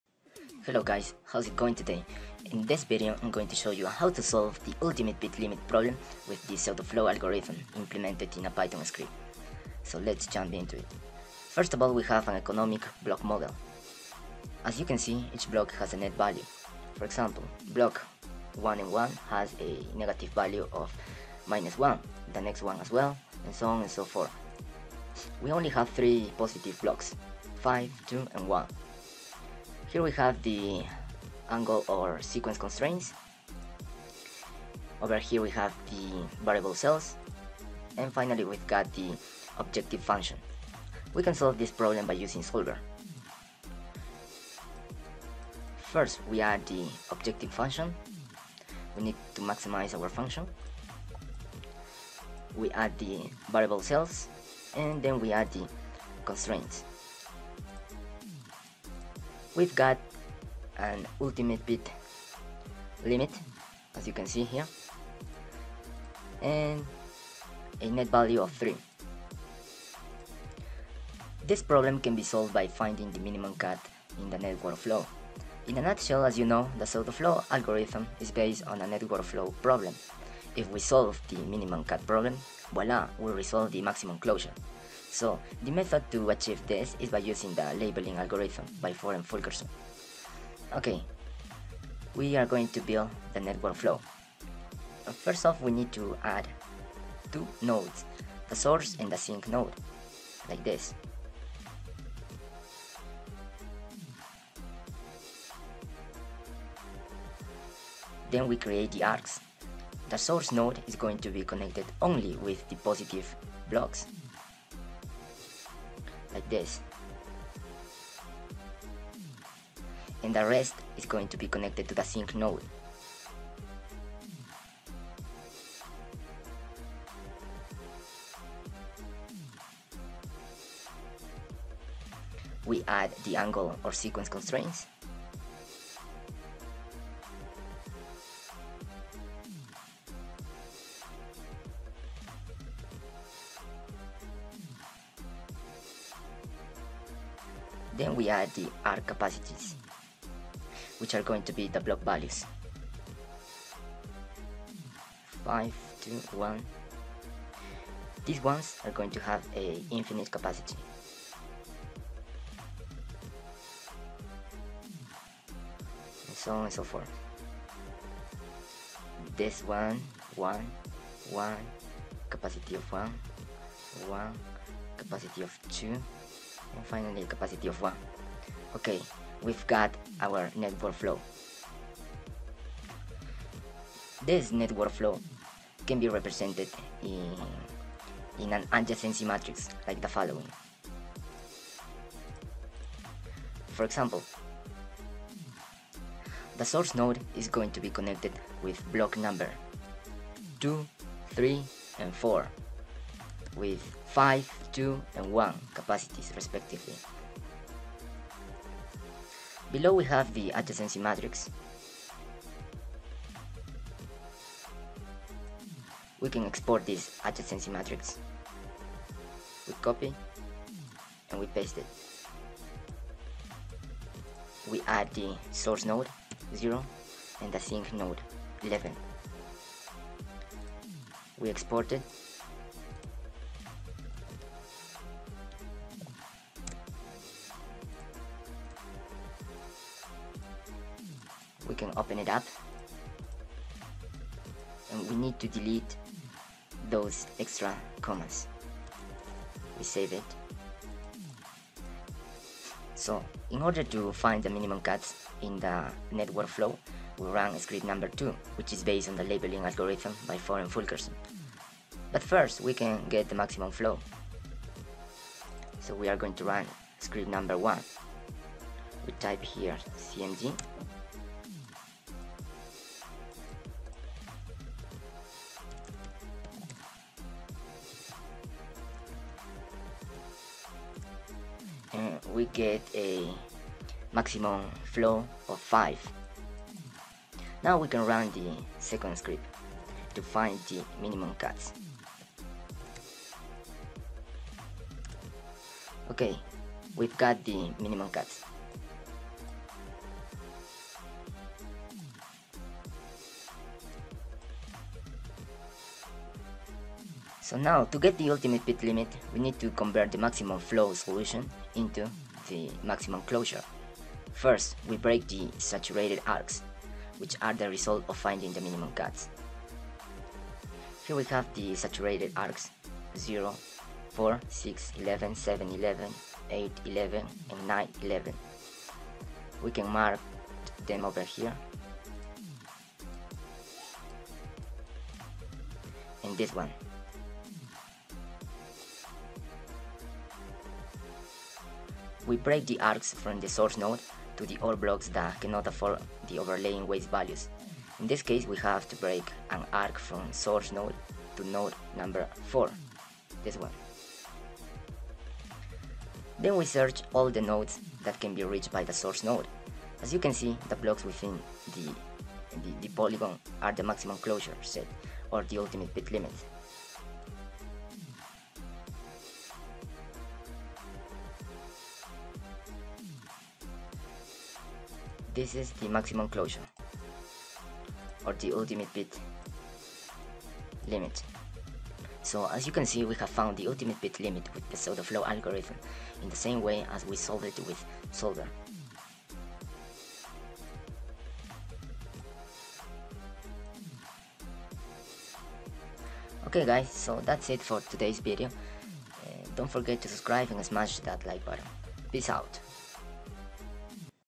Pseudoflow - Ultimate pit limit problem
How to solve the ultimate pit problem with an implementation of the pseudoflow algorithm in a python script.
The papers read:
- MAXIMAL CLOSURE OF AA GRAPH AND APPLICATIONS TO COMBINATORIAL PROBLEMAS, Jean Picard.
- MAXIMAL FLOW THROUGH A NETWORK, Ford and Fulkerson.
- PSEUDOFLOW METHOD FOR PIT OPTIMIZATION
- A LABELLING ALGORITHM FOR THE MAXIMUM-FLOW NETWORK PROBLEM
- THE PSEUDOFLOW ALGORITHM: A NEW ALGORITHM FOR THE MAXIMUM FLOW PROBLEM, Doris S. Hochbaum